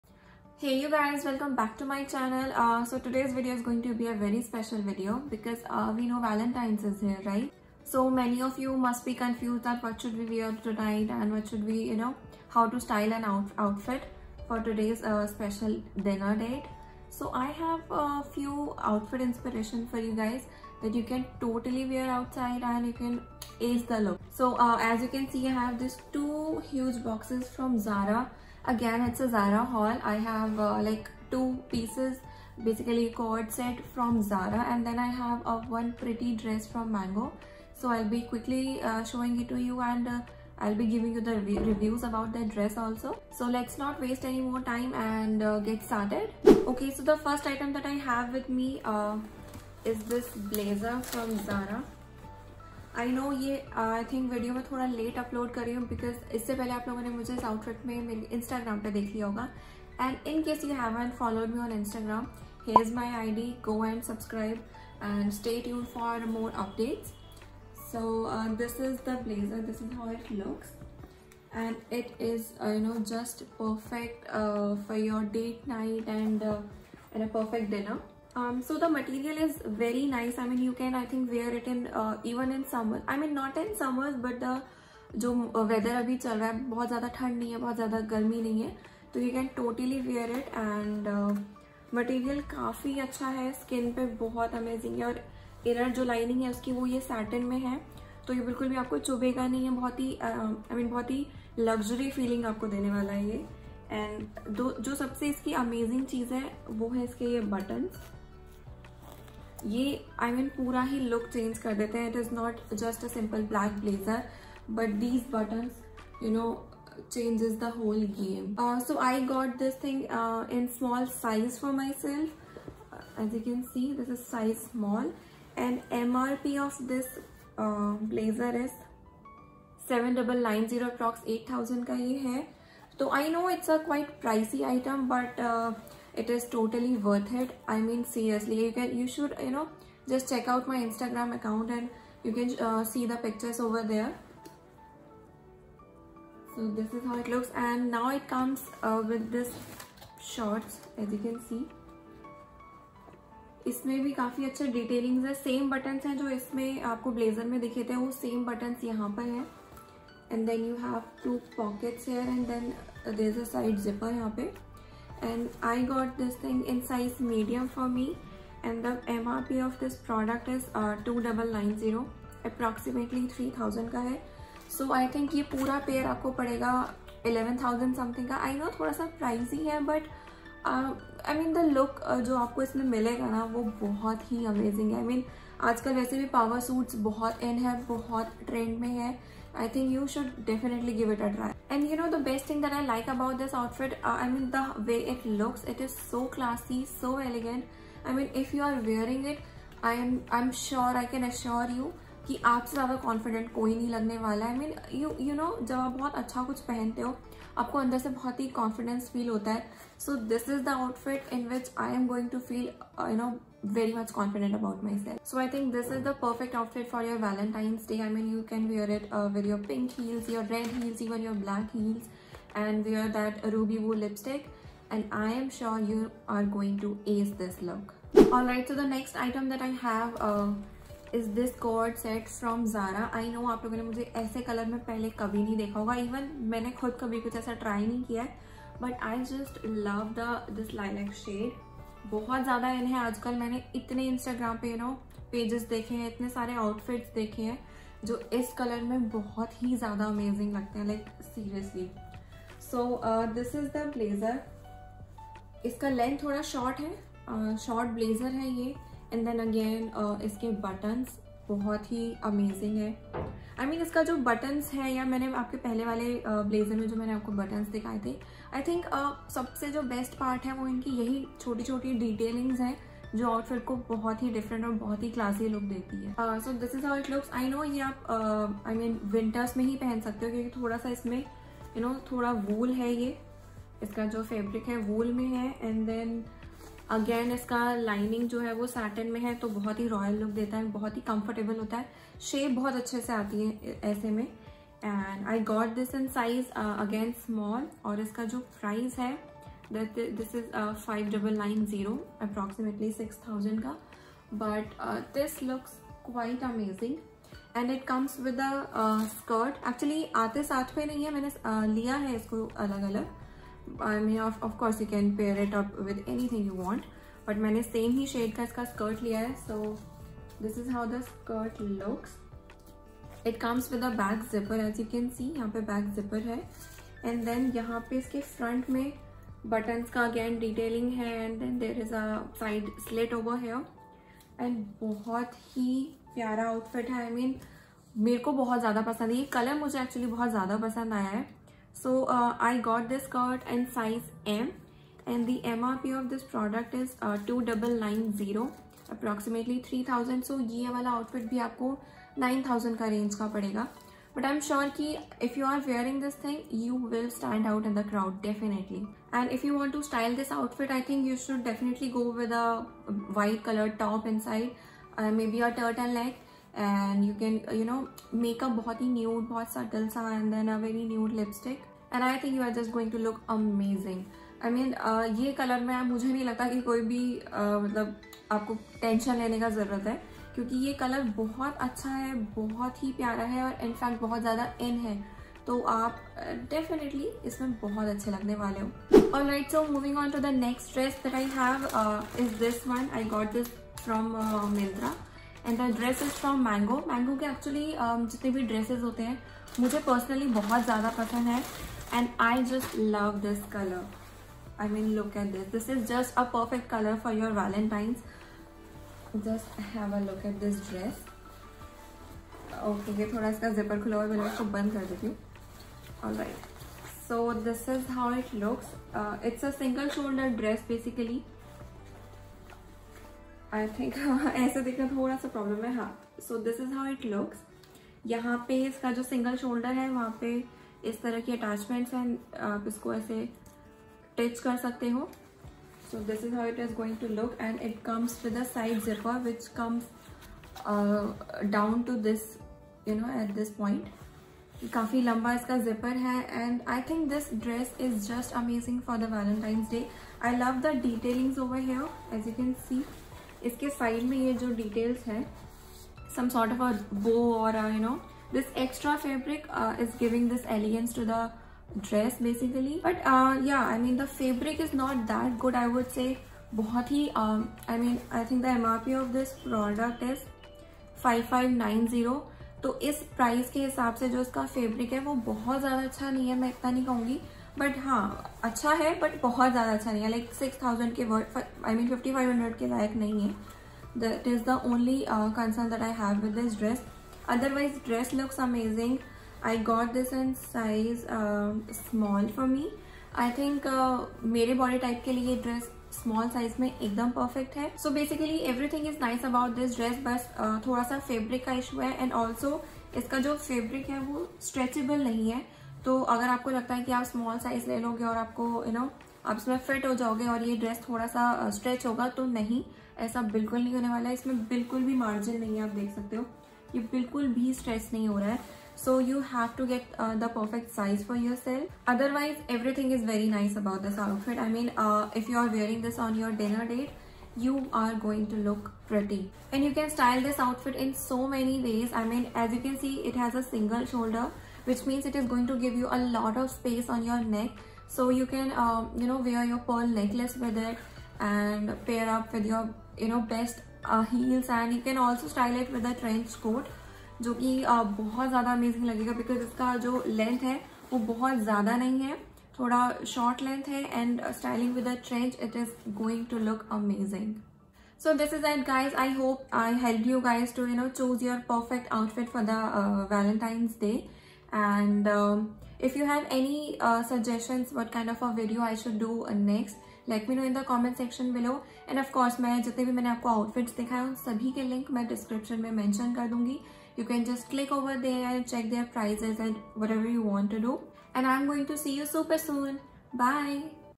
Huge Zara Haul Valentine's Collection 2021| Try on | What to wear on Valentine's Day| Ayushi Tyagi
I hope you all liked the video

Pink Dress - MANGO Women Pink Bow Detail One-Shoulder Sheath Dress